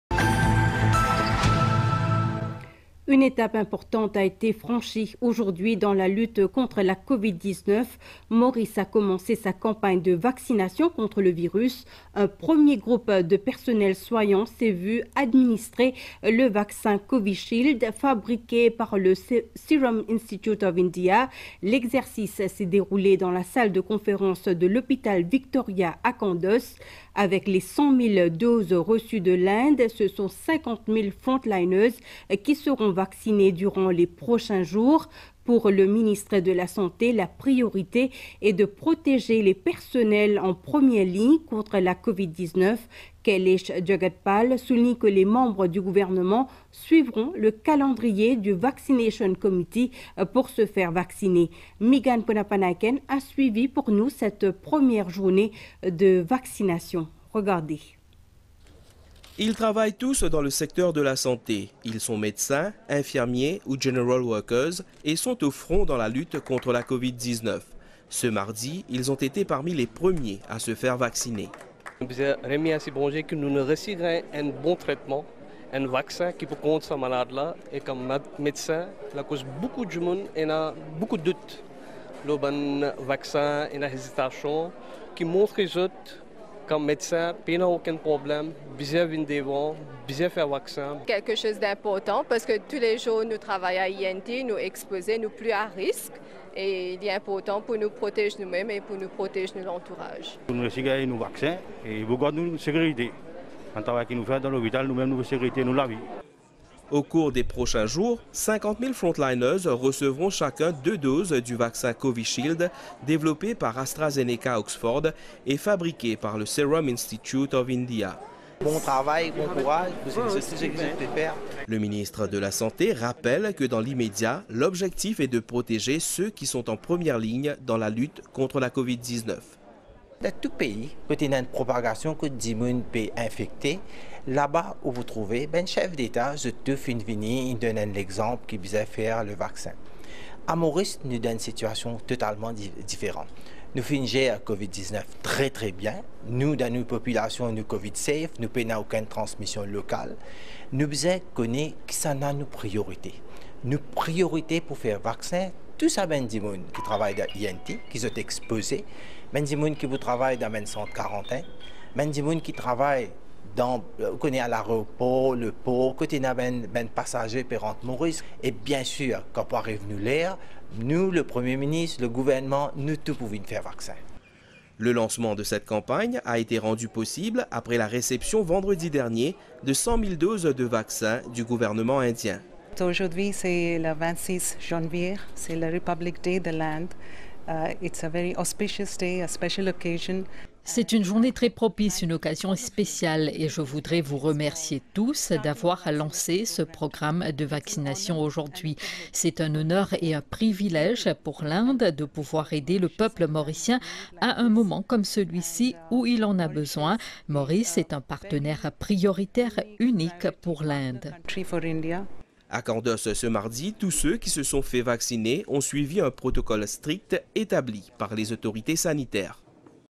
Mauritius launches Covid vaccination campaign with India-gifted vaccines | Le Journal 26.01.21
Mauritius launches Covid vaccination campaign with India-gifted vaccines

Source : MBC